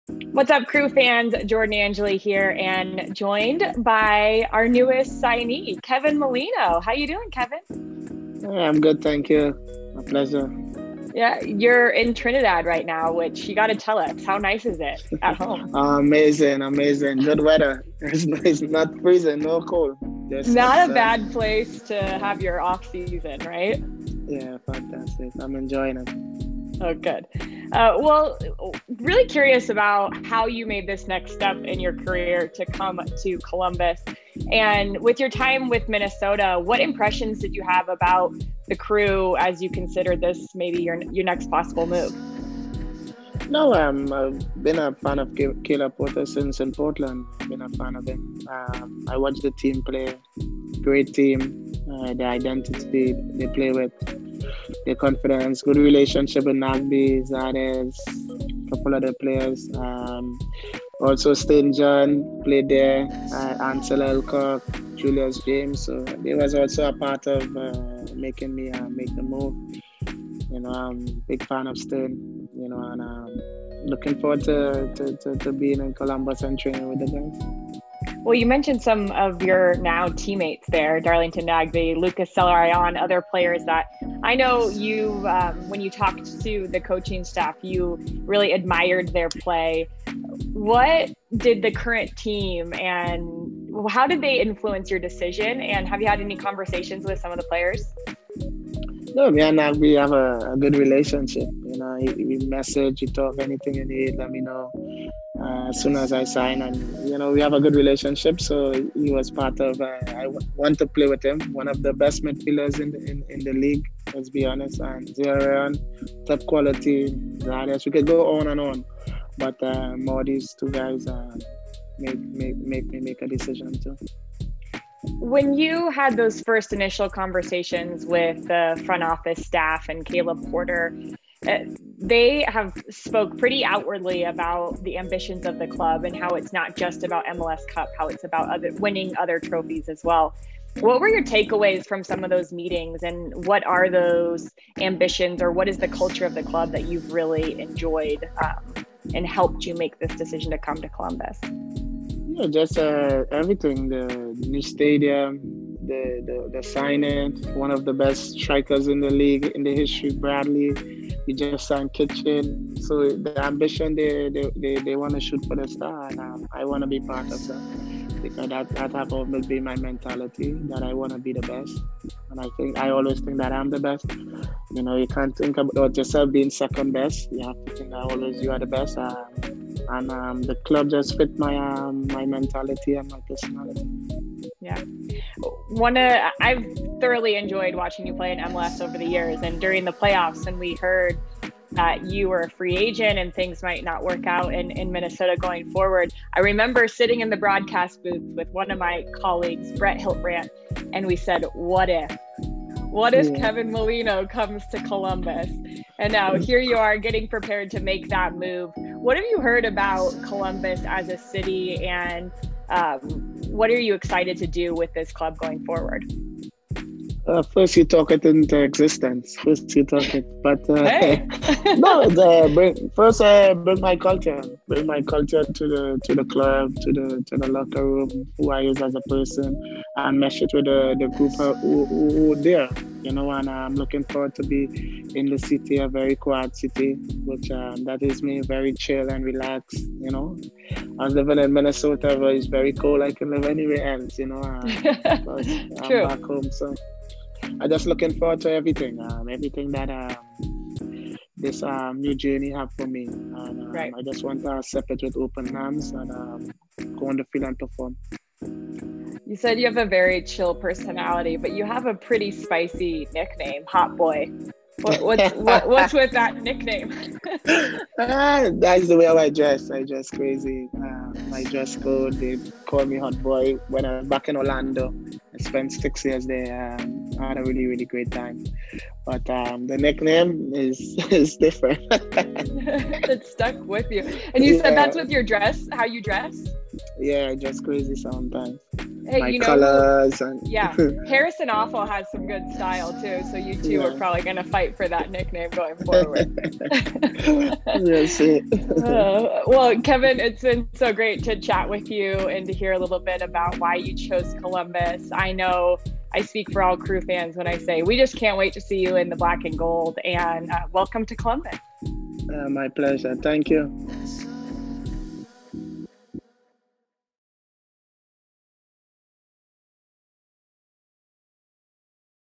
INTERVIEW | Kevin Molino's first interview as a member of the Crew
Jordan Angeli and Kevin Molino talk about what drew the free agent midfielder to Columbus as part of Molino's first interview since joining the Crew in January.